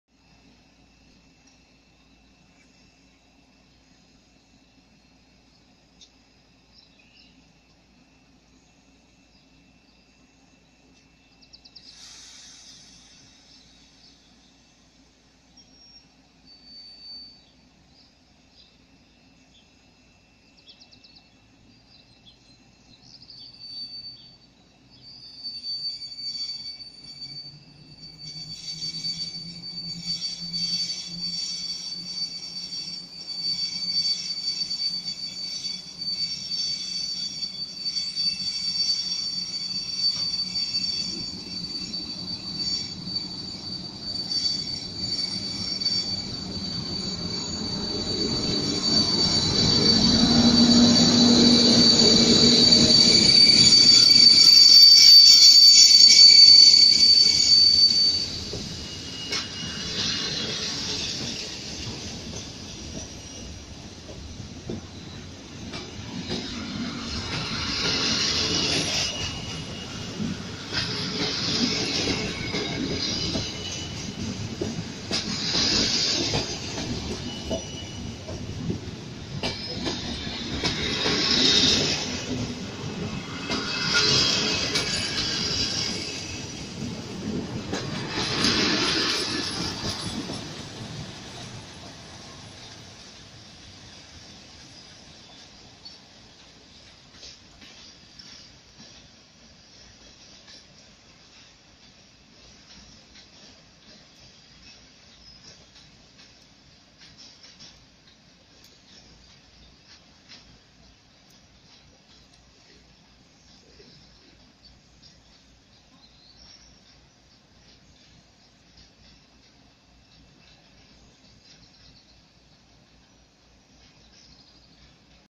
Забыл отпустить тормоз. Электровоз EU07-015 с поездом Интерсити Пшемышль - Краков.
Электровоз EU07-015 с поездом Интерсити, сообщением Пшемышль - Краков (отправление со станции Пшемышль, Польша)